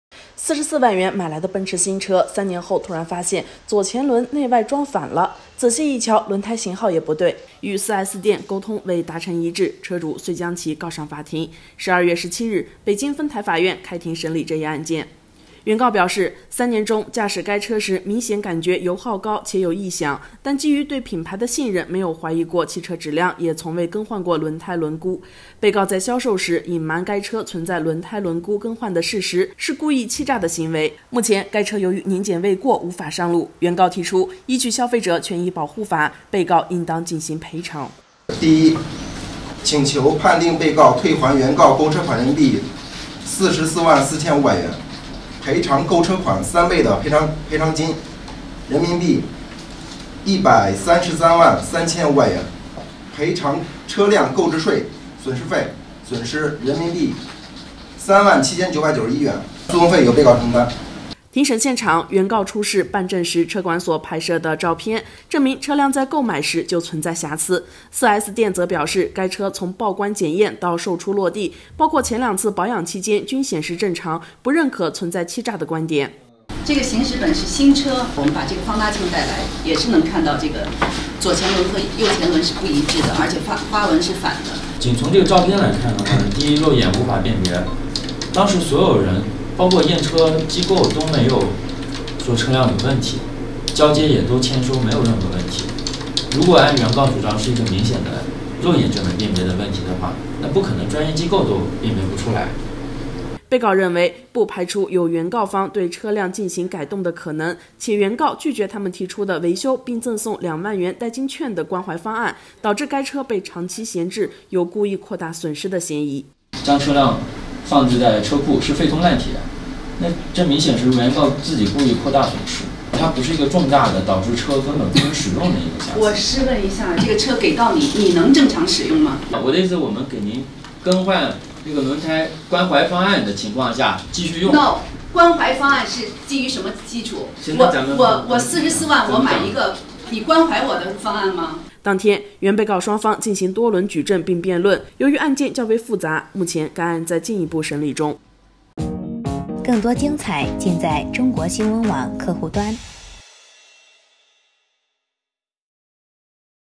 44万买的奔驰车，轱辘竟然是反的？车主起诉维权
44万元买来的奔驰新车，3年后突然发现左前轮内外装反了，仔细一瞧，轮胎型号也不对。与4S店沟通未达成一致，车主遂将其告上法庭。12月17日，北京丰台法院开庭审理这一案件。

原告表示，3年中，驾驶该车时明显感觉油耗高且有异响，但基于对品牌的信任没有怀疑过汽车质量，也从未更换过轮胎、轮毂。被告在销售时隐瞒该车存在轮胎、轮毂更换的事实，是故意欺诈的行为。目前该车由于年检未过无法上路，原告提出，依据消费者权益保护法，被告应当进行赔偿。